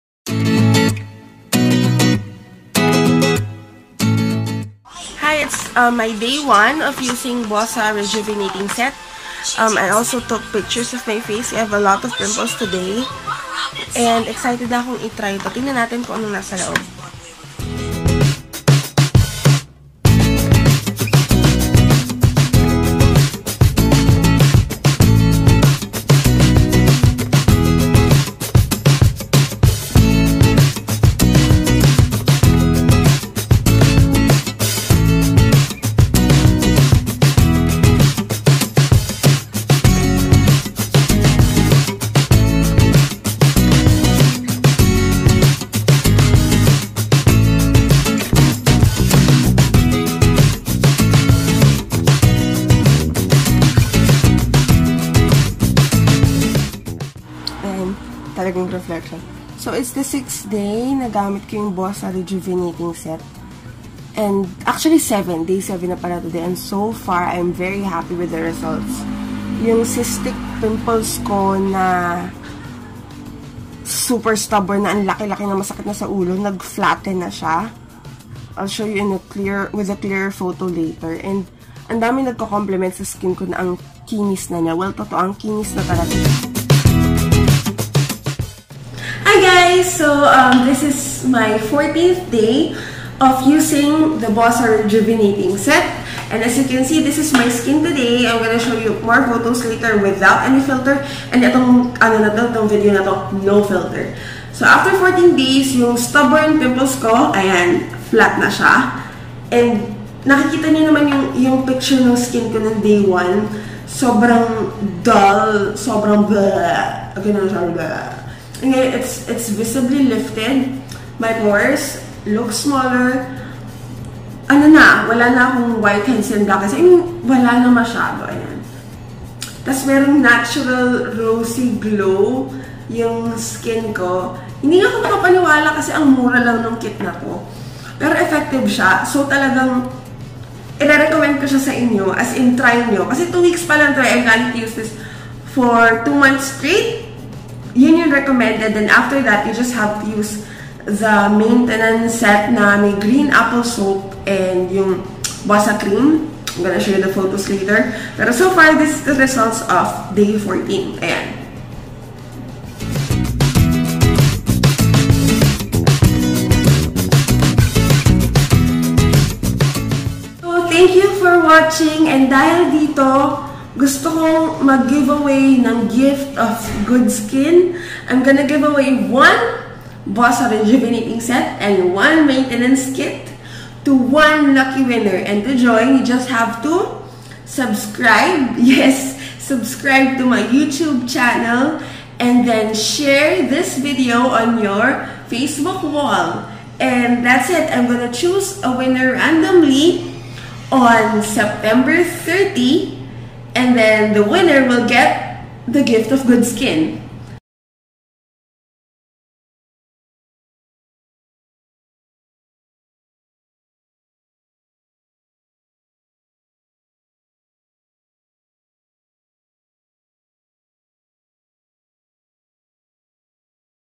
KVLOG #2: HOW MY SKIN GOT BETTER IN 2 WEEKS + GIVE-AWAY!
Hello there! Thanks for dropping by my video. Last August, I've been bombarded with a lot of cystic acne, redness, and dull skin. Being the curious and experimental guinea pig that I am. I went ahead and tried a new skincare system.

I'll be choosing and announcing the winner on September 30 2017

Good luck and may you all be blessed with good skin!